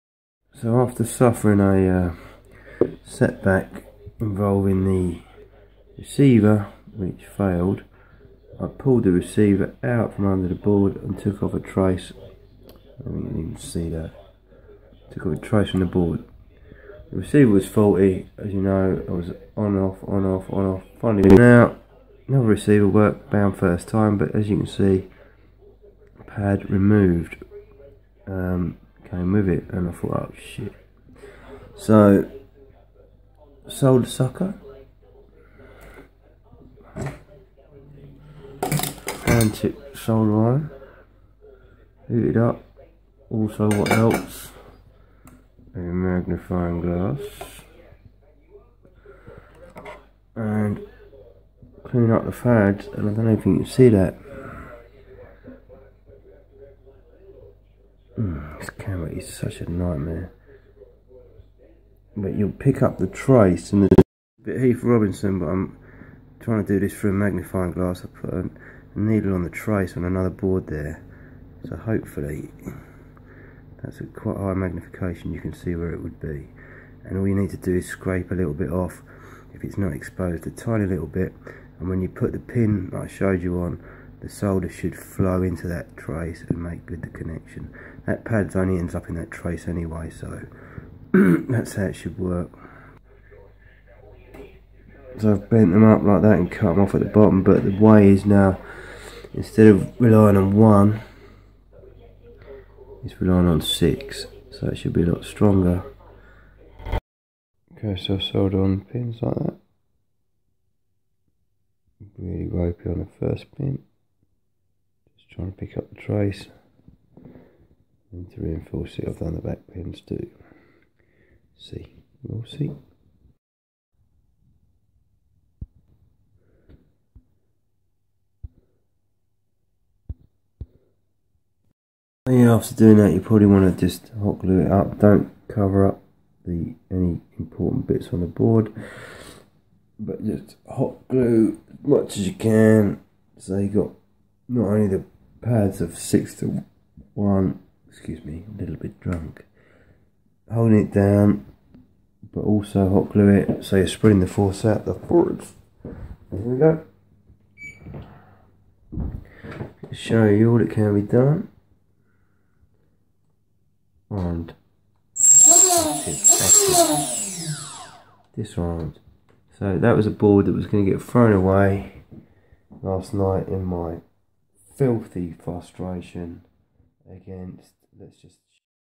Fix Damaged Solder Pad on Naze32 or other Flight Contoller
How to repair & re-inforce missing/lifted/damaged solder pads on Naze32.

Uses only solder, needle and a few header pins. Especially useful if cppm is desired/required and pin (1) is damaged (eg if RSSI or GPS is needed) or just if you want to save a damaged board... Part of full build video of weather resistant Blackout Mini H with GPS, OSD, decent loiter and RTH.